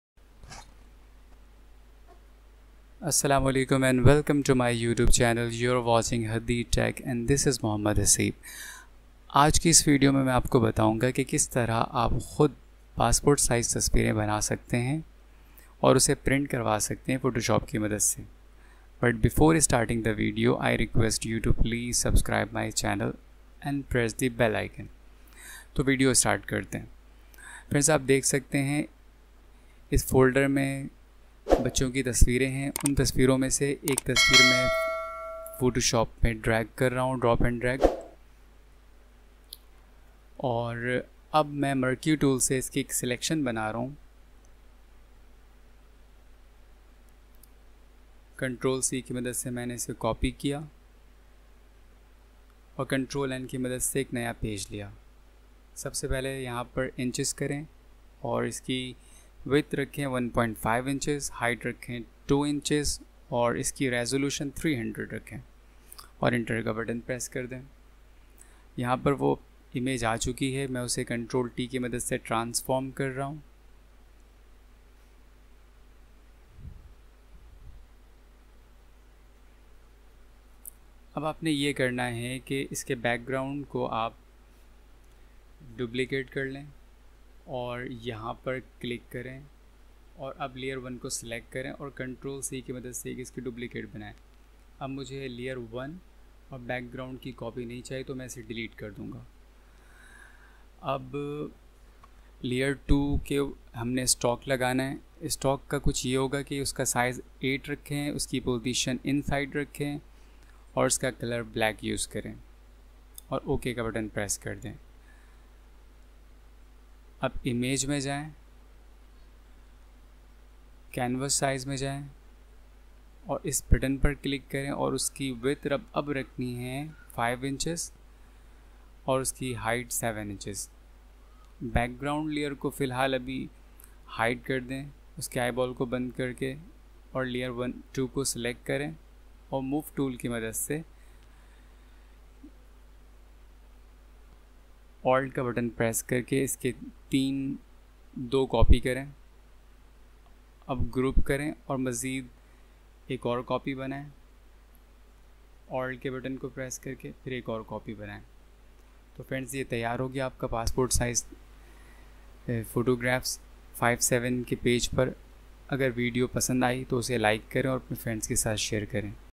Passport Size Photos | Adobe Photoshop | Hadeer Tech
in this tutorial we will learn how to make passport size photos in adobe photoshop professionally in just a few steps. watch the video and follow the steps.
if you want to ask anything please comment.